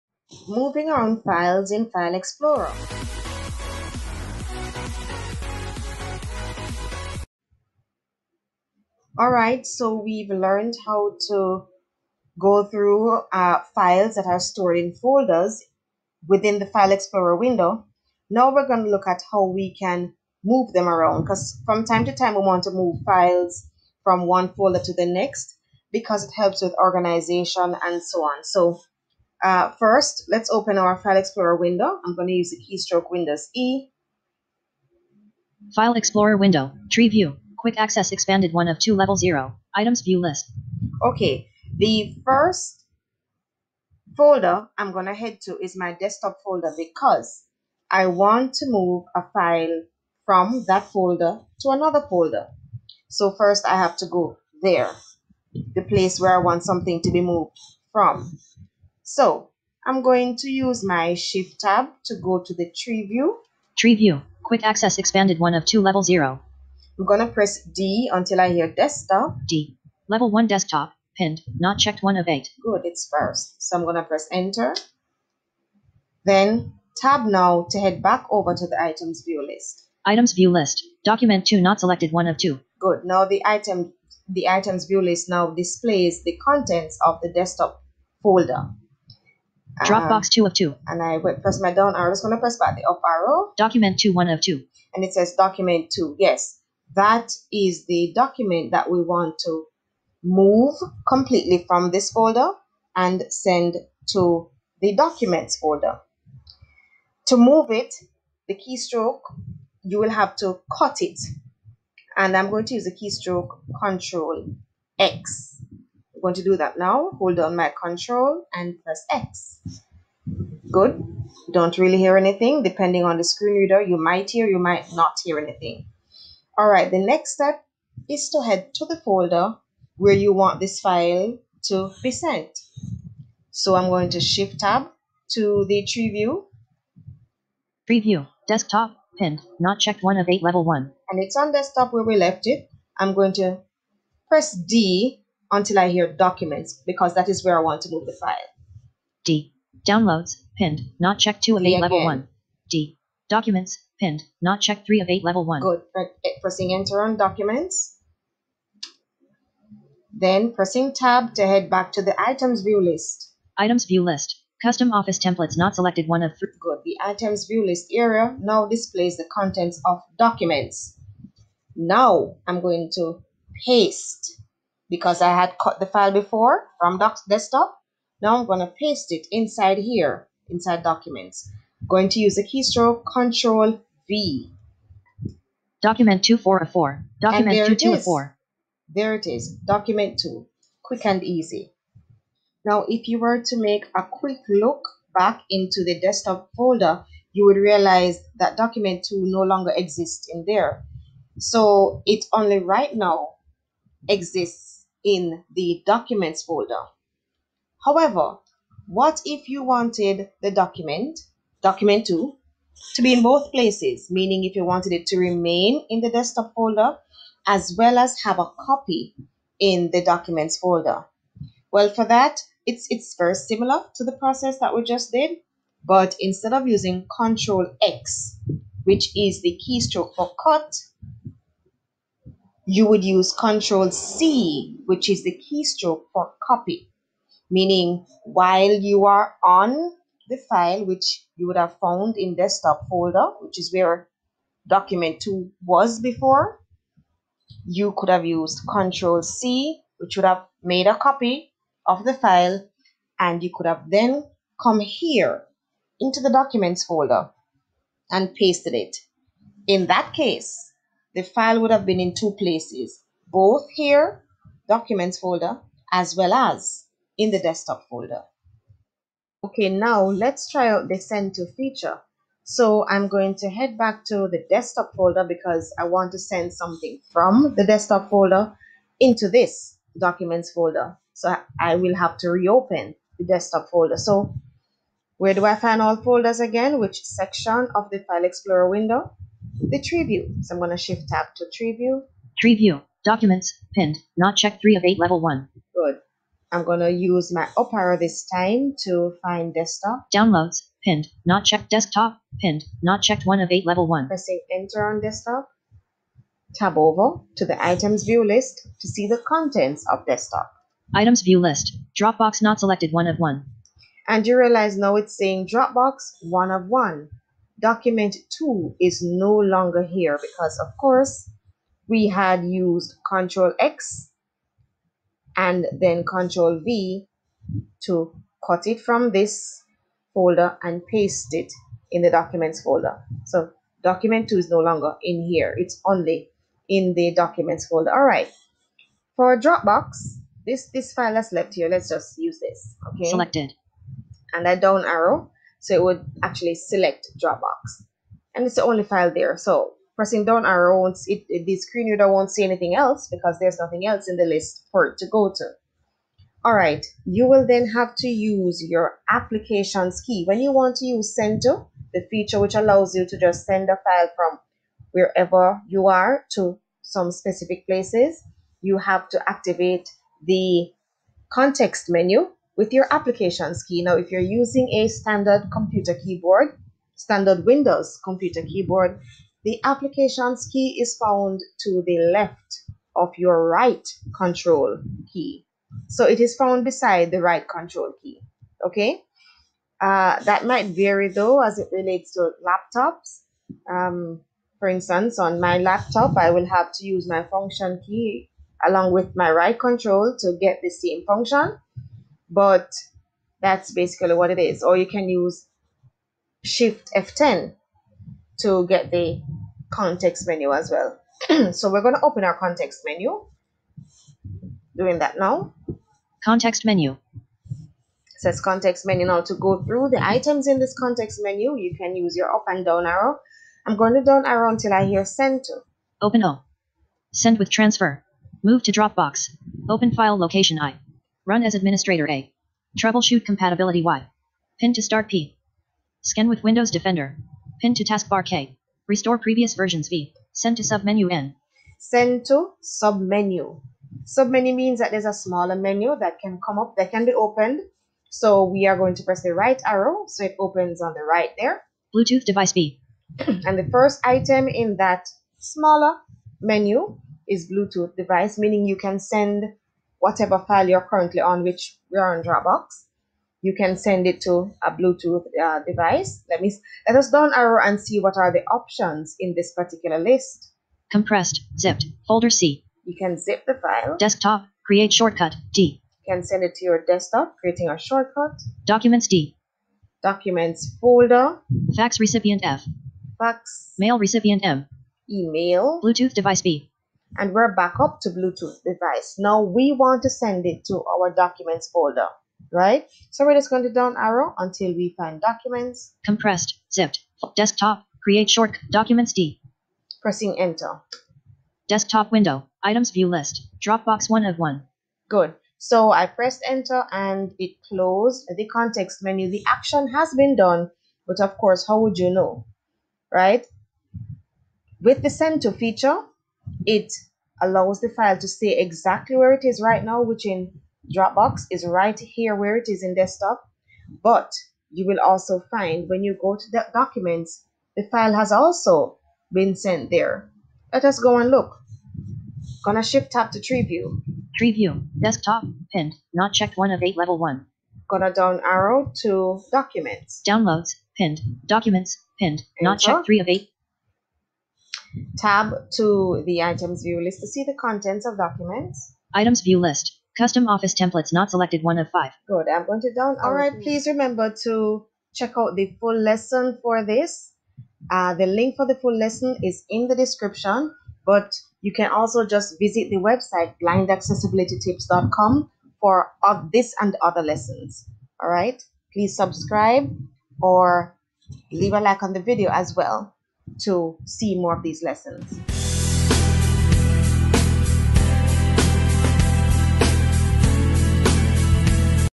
Moving around files in File Explorer with a screen reader
This video is intended for blind and visually impaired persons who use a screen reader to access the computer. It is also intended for their family members and teachers.

Watch this video to learn exactly how to shift files around in File Explorer on a Windows computer. Want to learn how to cut and copy files or use the "send to" feature in File Explorer to help organize your files? Then this is the video for you.

to see other lessons.